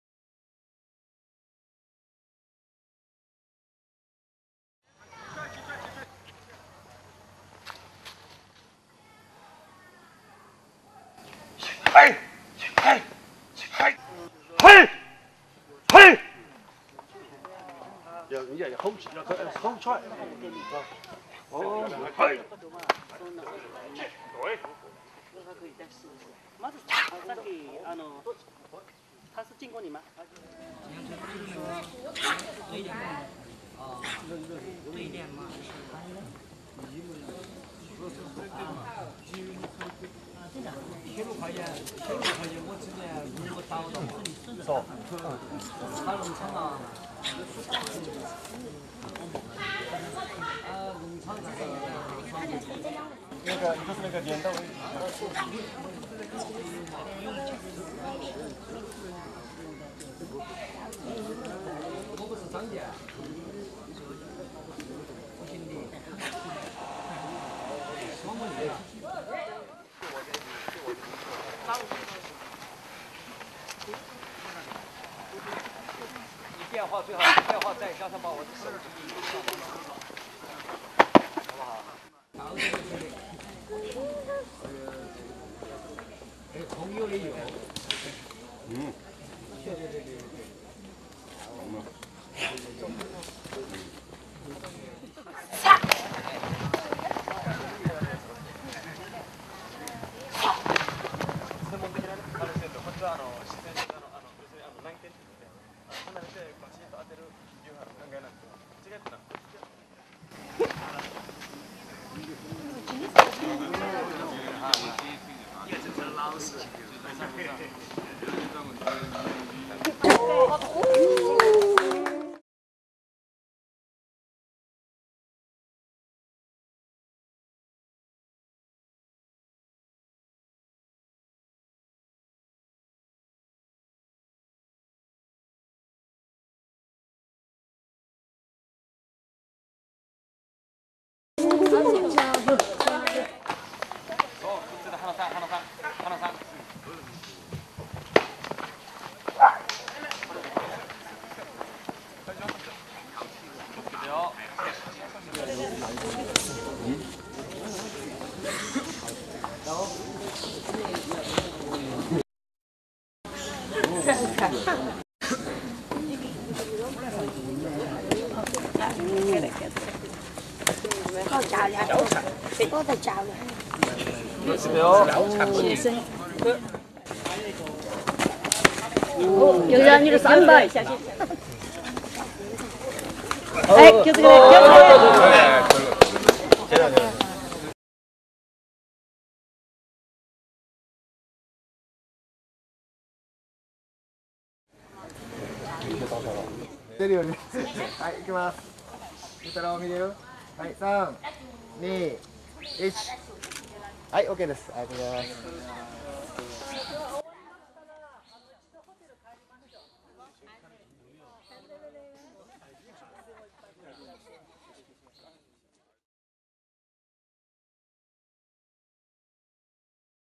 散手交流2007---天行健中国武術館
この交流会は以前より天行健中国武術館館長・宮平先生に私淑されていた四川省六合門の温　成燕氏が、師の陳　義偉老師を紹介されたことにより実現した2007年度の訪中散手交流会の様子です。